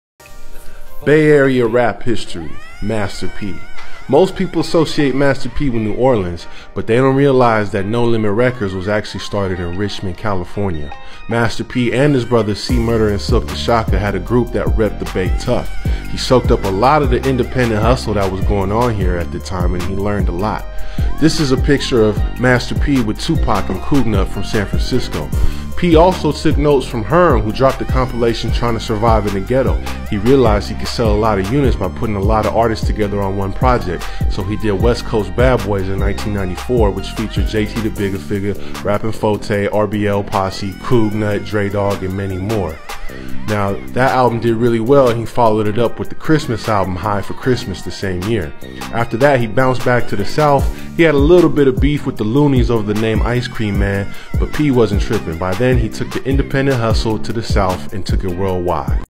History of Bay Area Rap: Master P
Master P and No Limit Records' origins in Richmond, CA.